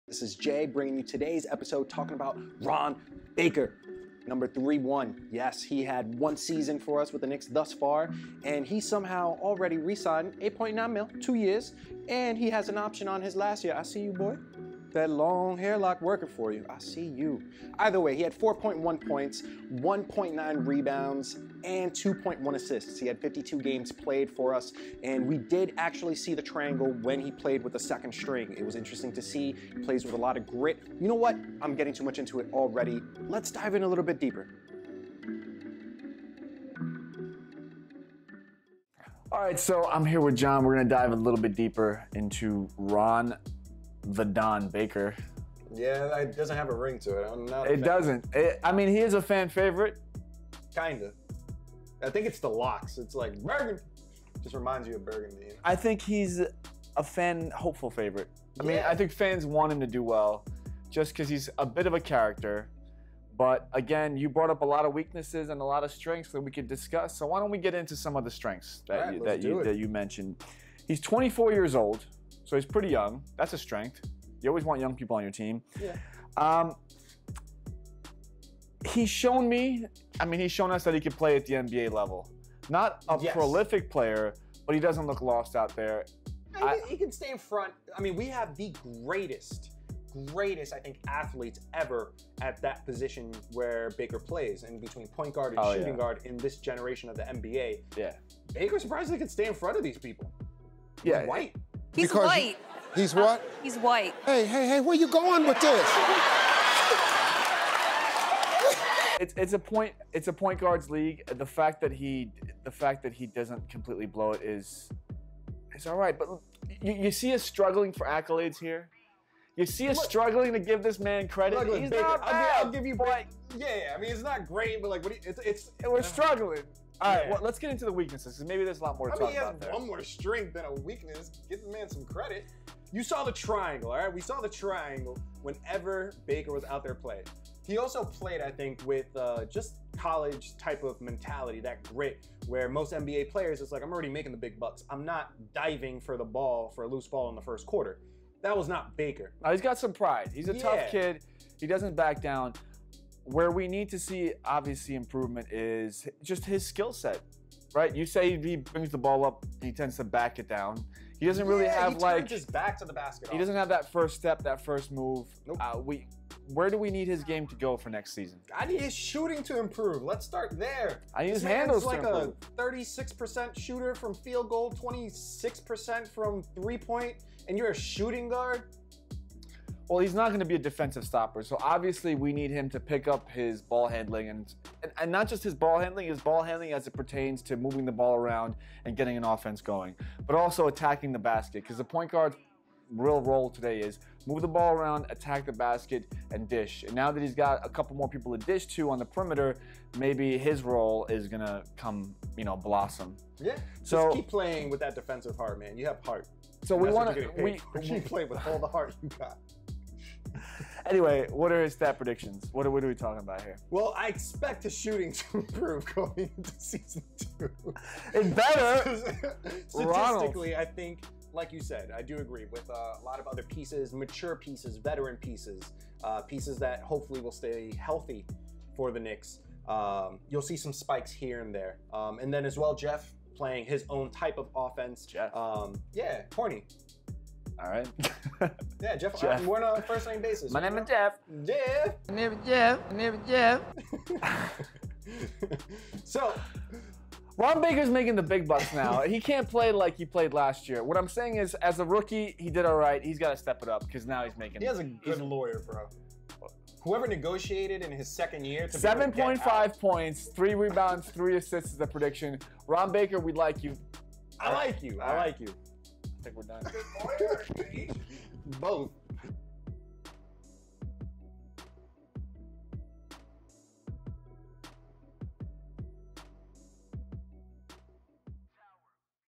Ron Baker 2018 Stat Predictions - MadGood Profiles
We welcome you to our second MadGood Profile as we chronicle the offseason and welcome in some old and new faces for the 2018 season. We speak on strengths and weaknesses and 2018 expectations. TUNE IN FOR OUR OFFSEASON EPISODES AS WE GEAR UP FOR SEASON 2!!!